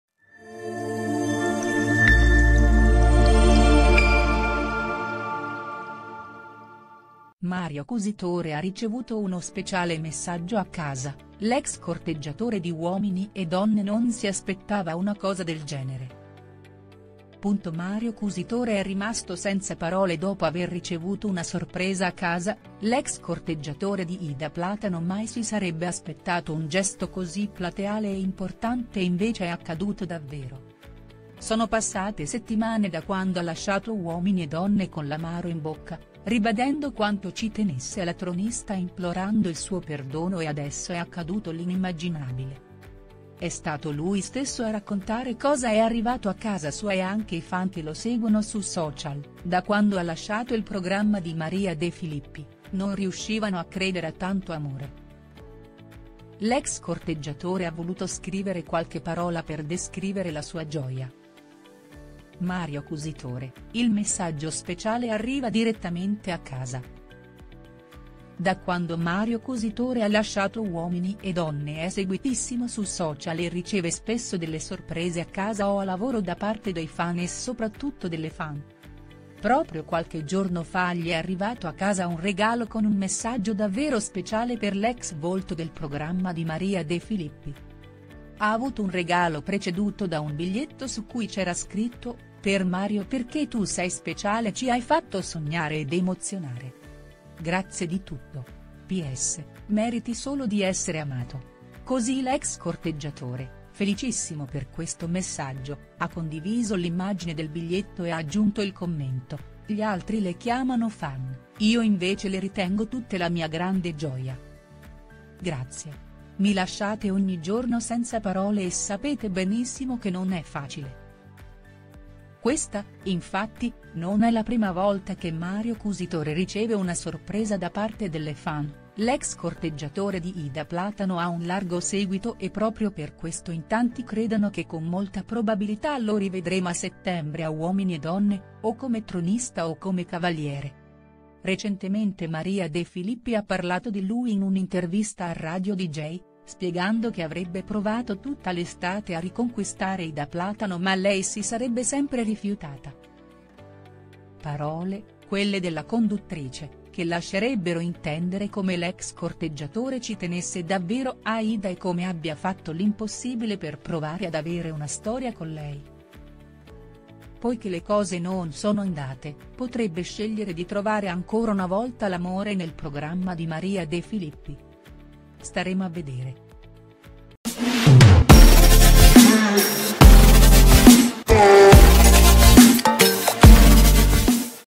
“Senza parole” sorpresa per Mario Cusitore, cosa è arrivato a casa dell’ex Uomini e donne
Mario Cusitore ha ricevuto uno speciale messaggio a casa, l’ex corteggiatore di Uomini e Donne non si aspettava una cosa del genere.

Mario Cusitore è rimasto senza parole dopo aver ricevuto una sorpresa a casa, l’ex corteggiatore di Ida Platano mai si sarebbe aspettato un gesto così plateale e importante e invece è accaduto davvero. Sono passate settimane da quando ha lasciato Uomini e Donne con l’amaro in bocca, ribadendo quanto ci tenesse alla tronista e implorando il suo perdono e adesso è accaduto l’inimmaginabile.